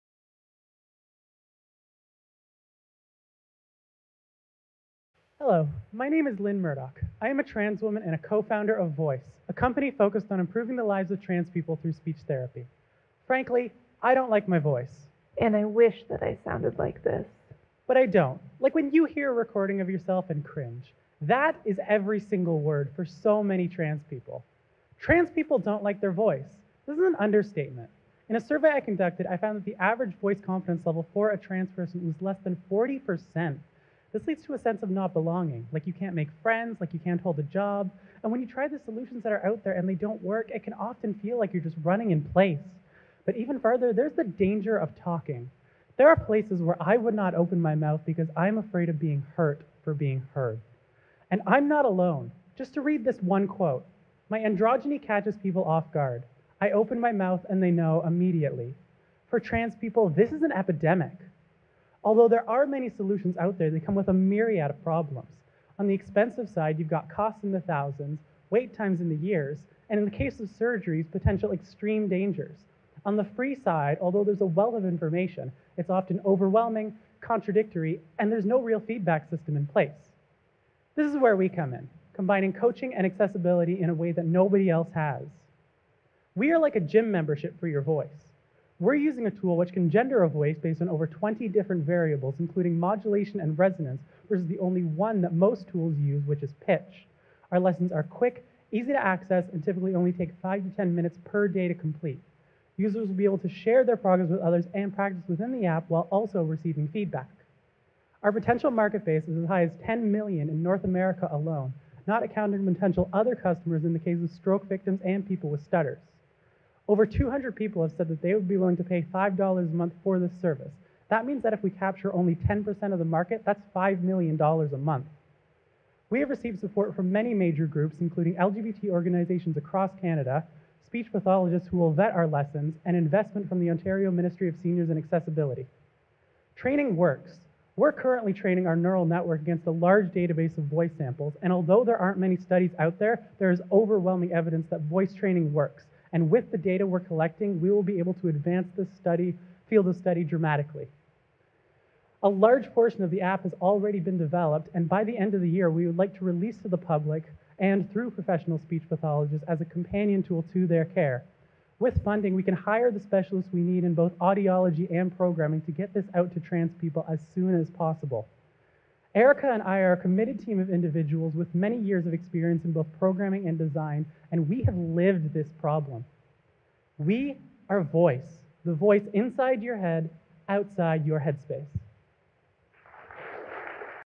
VOYHS
Lynne Murdoch pitches for VOYHS, they give trans people the resources to train their voice in order to boost their confidence and improve their lives.
VOYHS is a $5K winner!